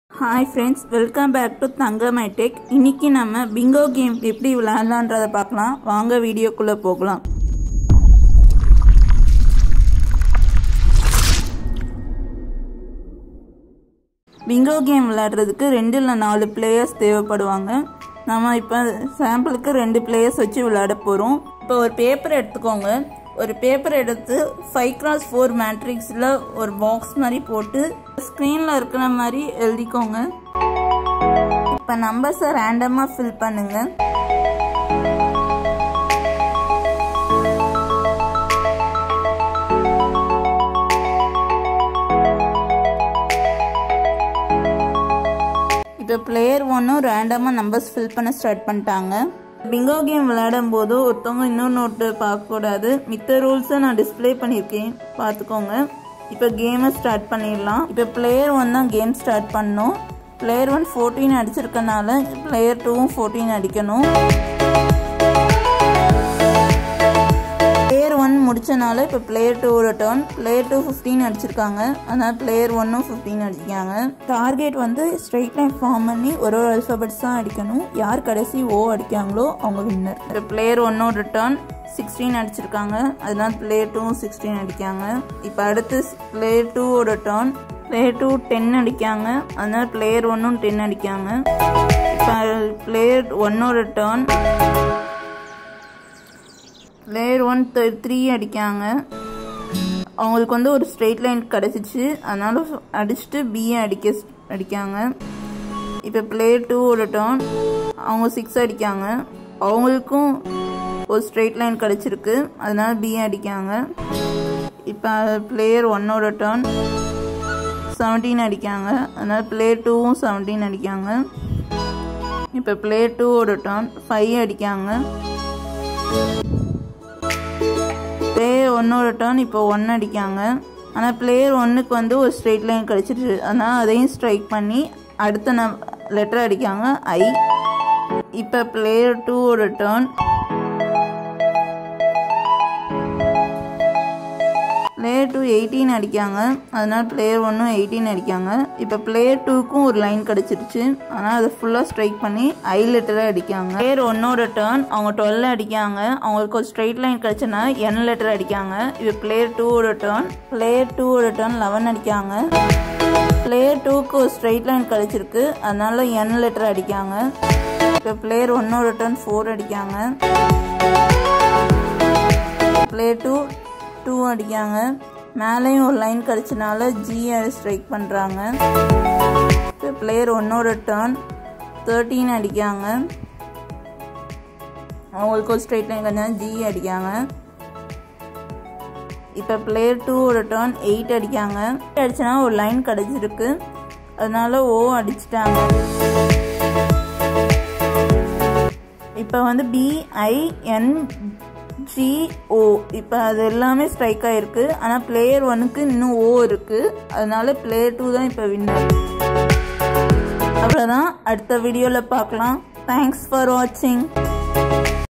How to play bingo game in tamil
Note: player 1 number 2 va adikkavam instead of leaving it..( typo error)

In the video, we are going to see about bingo game.
Rules:
1) Hidden the number from opposites team mates.
2) Cross the lines such as Diagonal , Horizontal, Vertical etc...
3) Complete the word- "BINGO" first is the winner.